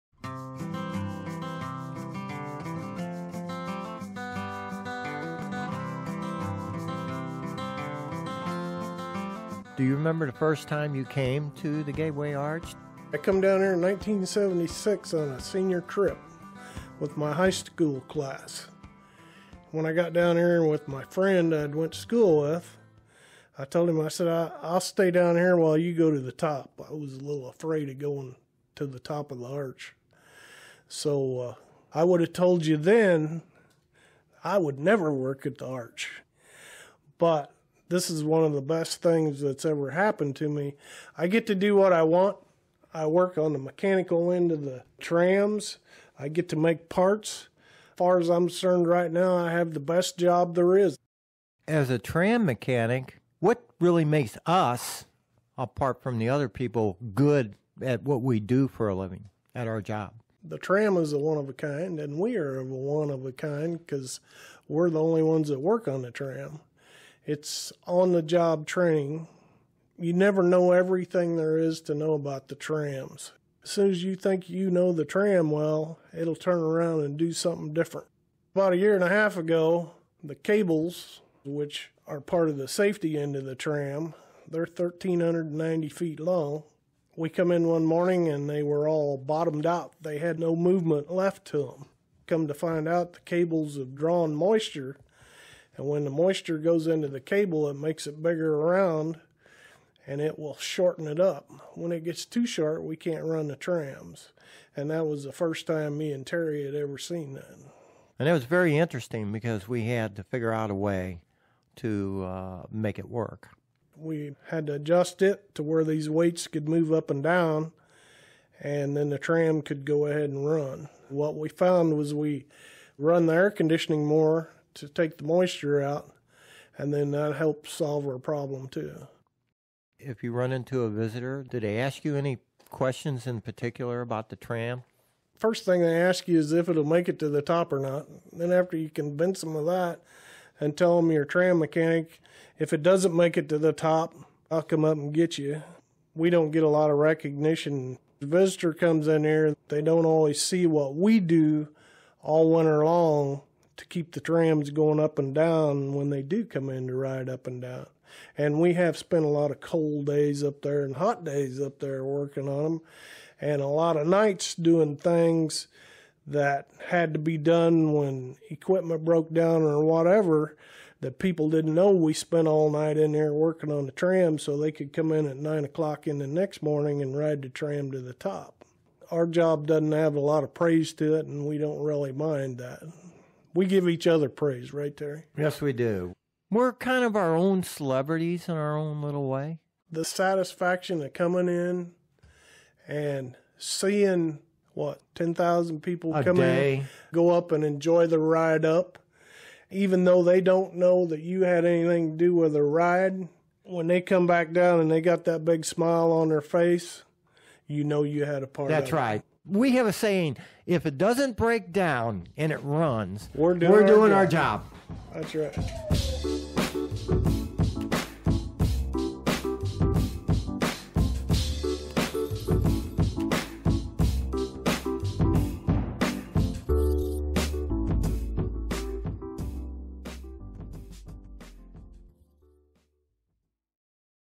StoryCorps: Jefferson National Expansion
Roger Smith and Terry DiBlasi are veteran mechanics at the Jefferson National Expansion Memorial in St. Louis - which includes the Gateway Arch. They work on the tram that takes visitors up and down the 630 foot high Arch.